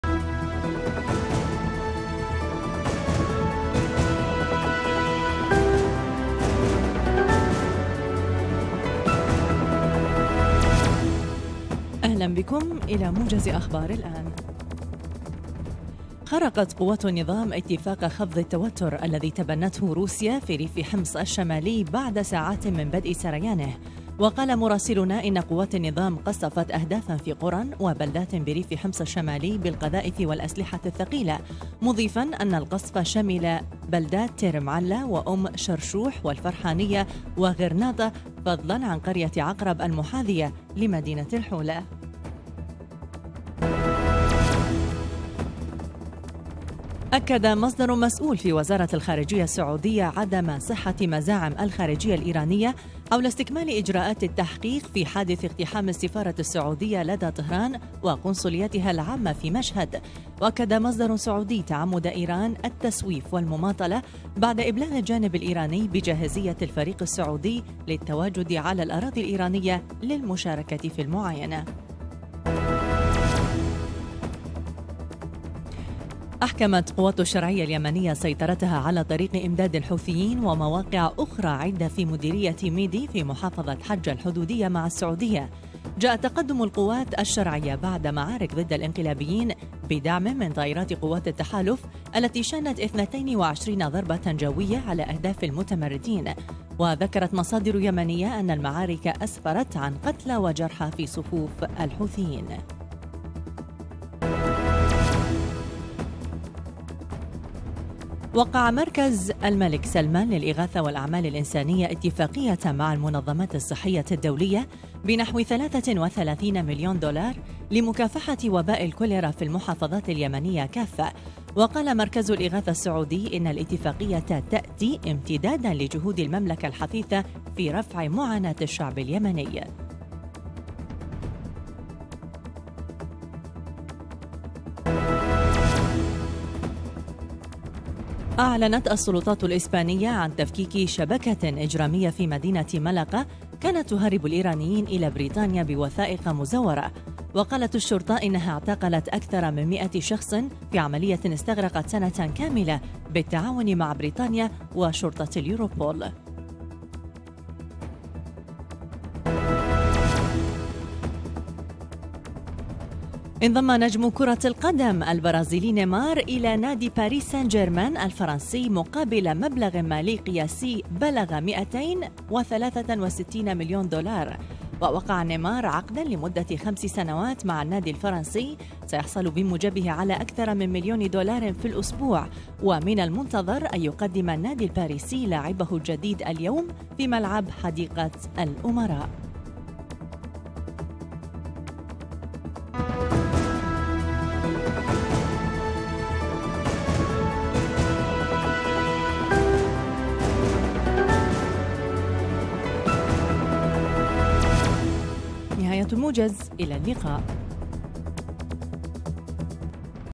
04-08-2017 | موجز التاسعة صباحاً.. لأهم الأخبار من
أبرز العناوين:
- قوات النظام تخرق الهدنة وتقصف قرى وبلدات بريف حمص الشمالي
- السعودية تنفي مزاعم إيران باستكمال تحقيقات اقتحام السفارة
- قوات الشرعية تسيطر على طريق إمداد لـ الحوثيين في ميدي
- السعودية تخصص عشرات ملايين الدولارات لمكافحة الكوليرا في
- إسبانيا تفكك شبكة إجرامية تهرب إيرانيين إلى